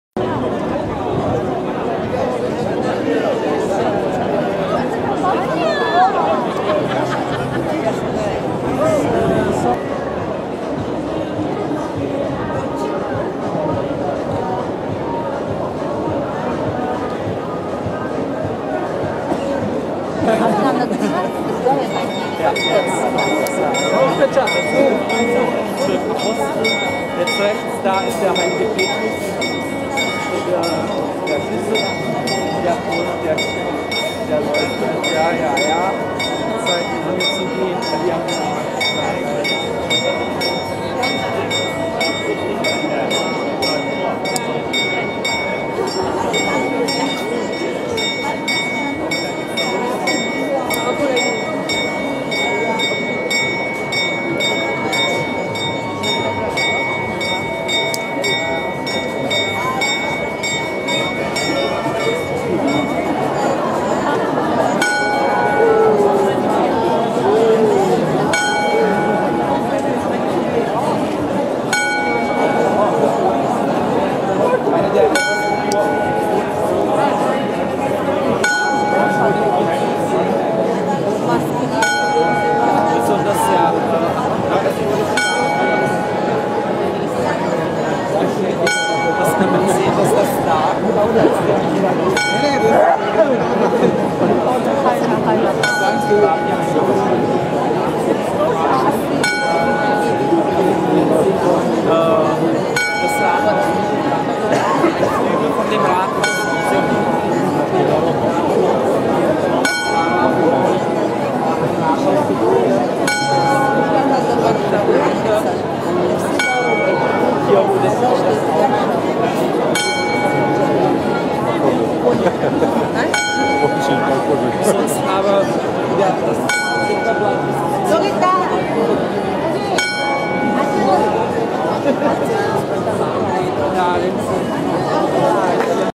CZ: Prague. Astronomical Clock Chiming by Night. April 2017
Prague. Astronomical Clock Chiming by Night. April 2017
Astronomische Uhr am Prager Rathaus zur vollen Stunde abends

Meine Kamera
Meine Dashcam
Meine Speicherkarte